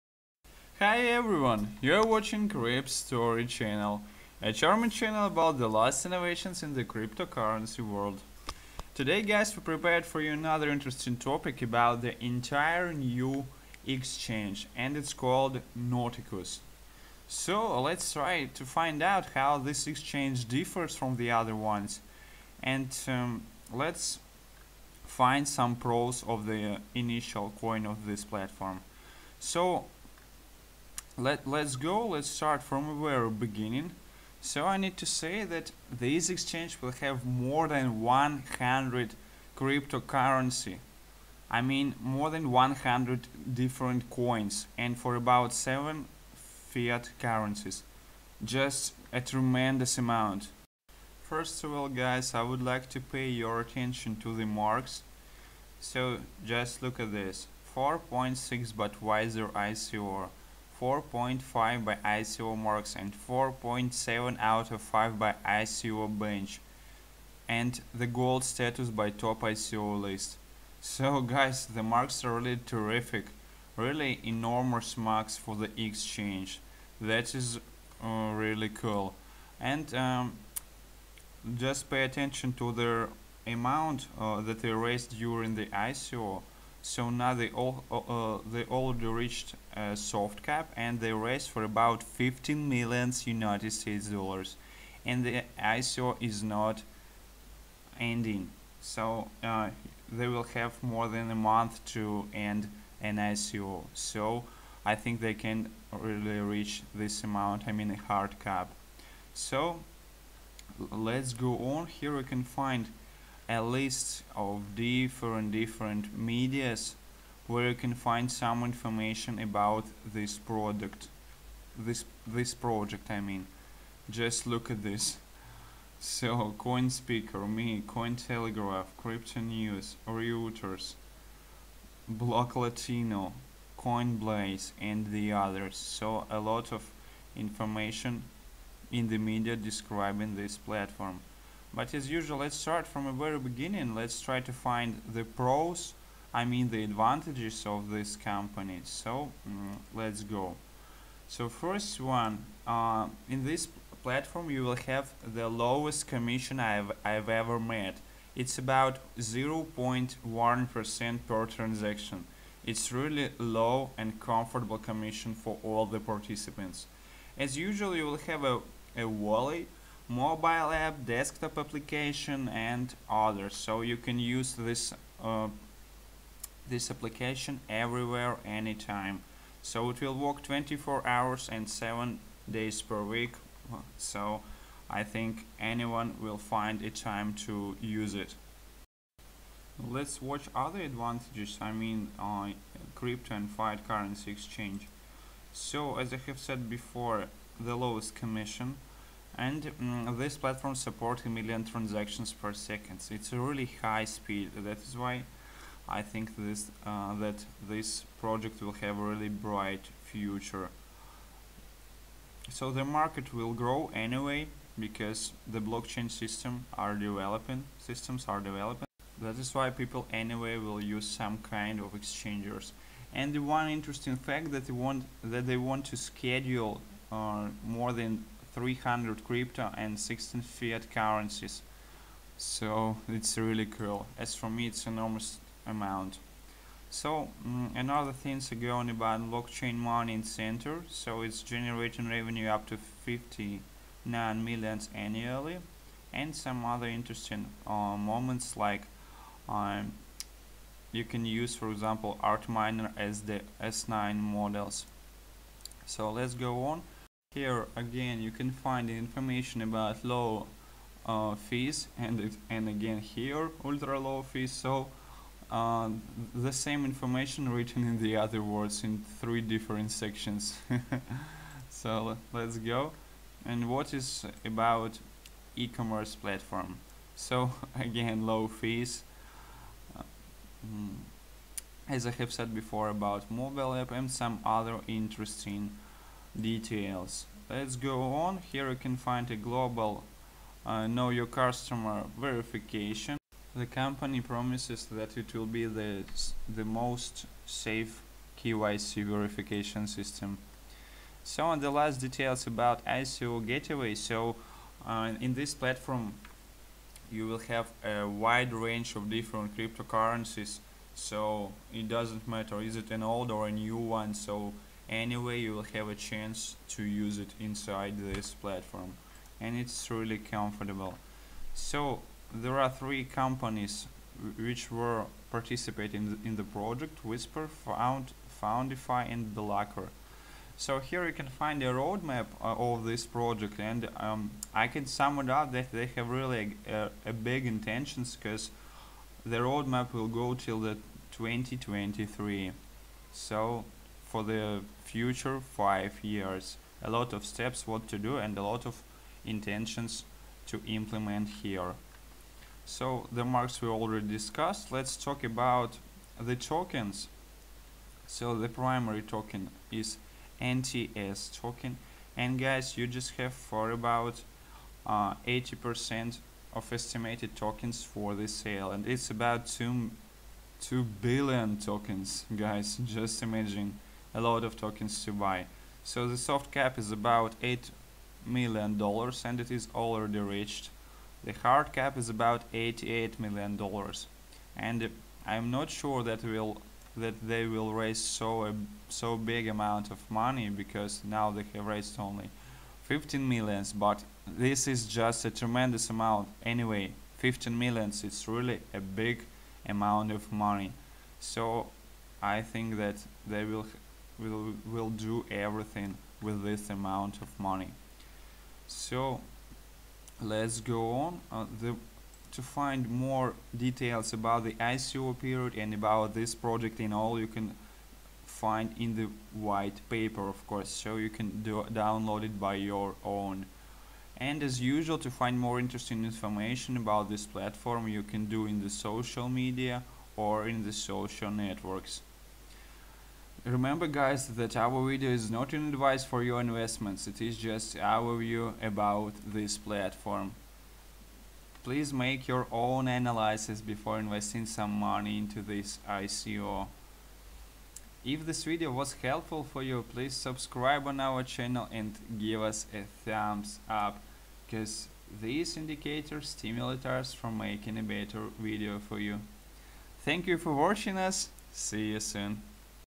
Nauticus - One coin for six global banking and eCommerce solutions
The Nauticus Exchange is designed to be the only
exchange users ever need — a one stop shop offering
300 cryptocurrencies and 16 fiat. Users from around
the world will be able to buy whatever crypto they
wish using their own nation’s money.
The Exchange will also enable users and merchants
to send and receive payments internationally — like
lower fees and live market rates. More than 200,000
users have already signed up to the platform and
Nauticus is registered with Australian Government
regulator AUSTRAC